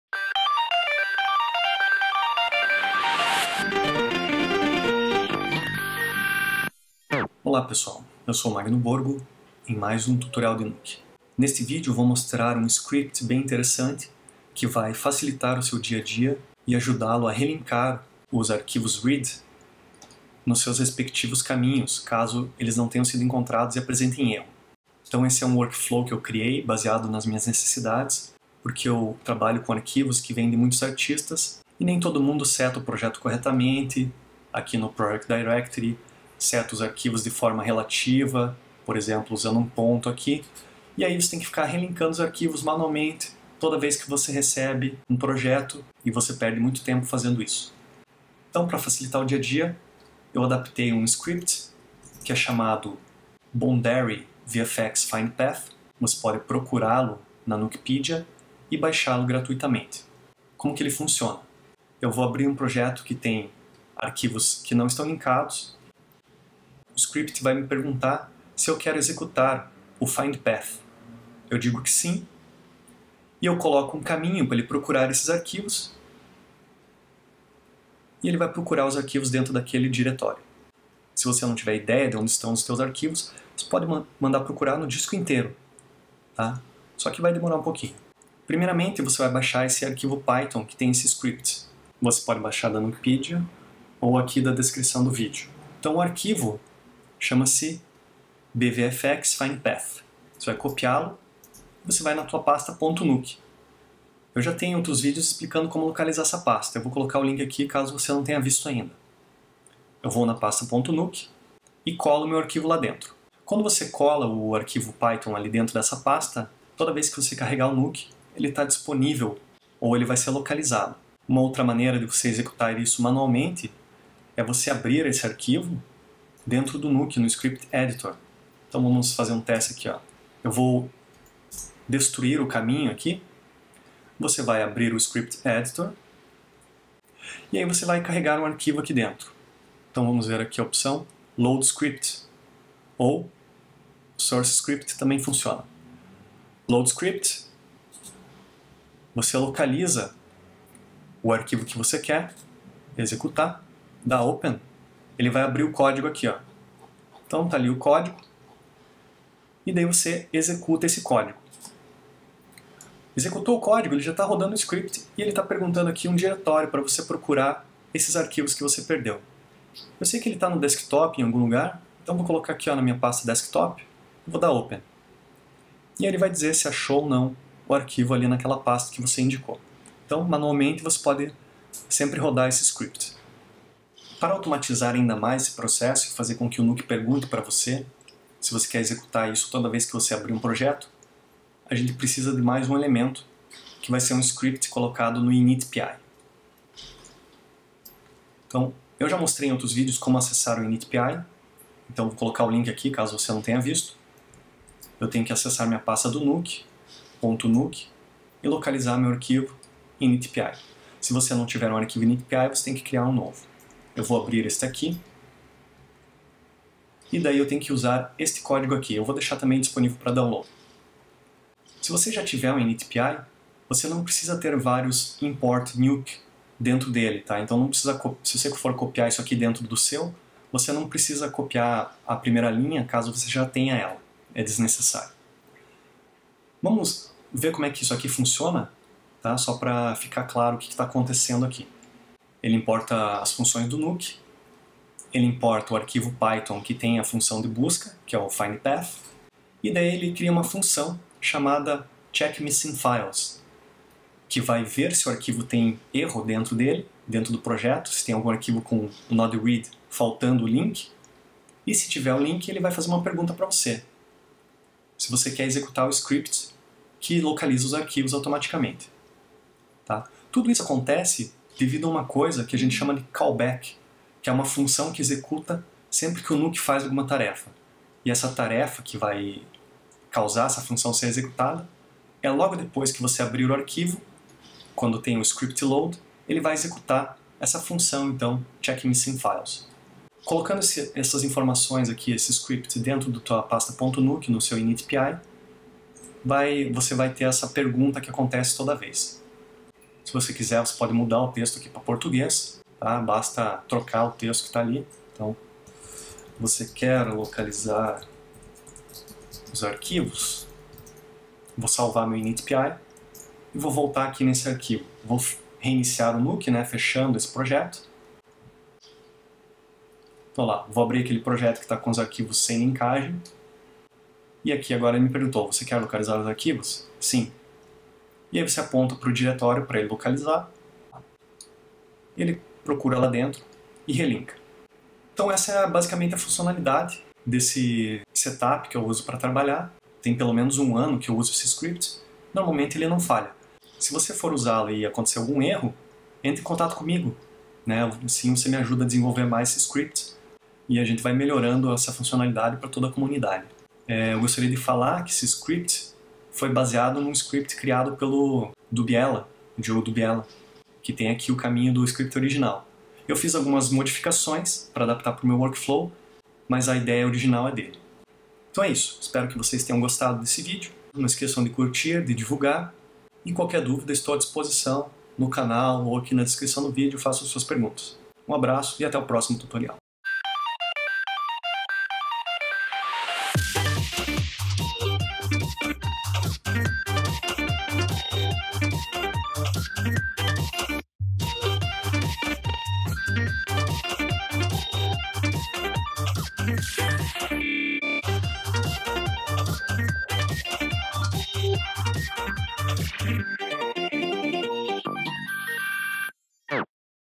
Nuke - Avançado - Python: bvfx_FindPath (Curso de Nuke em português)
Chega de ficar re-linkando o caminho dos arquivos manualmente nos nodes Read.
Neste vídeo mostro um workflow que uso para automatizar esta tarefa infame e que consome nosso precioso tempo.

Tutorial em português para o software Nuke (the Foundry) por Magno Borgo, treinador certificado Nuke.

Tópicos abordados:

Demonstração do script 0:48
Instalação 1:28
Executando o script pelo Script Editor 2:04
Automatizando o script para rodar sempre que um projeto
com erros for aberto 3:04

On this video I show how to relink file paths automagically using the bvfx_findPath script. Learn to speed up this boring everyday task!

Topics:

Script Demo 0:48
Instalation 1:28
Running from the Script Editor 2:04
Further automation using a python callback 3:04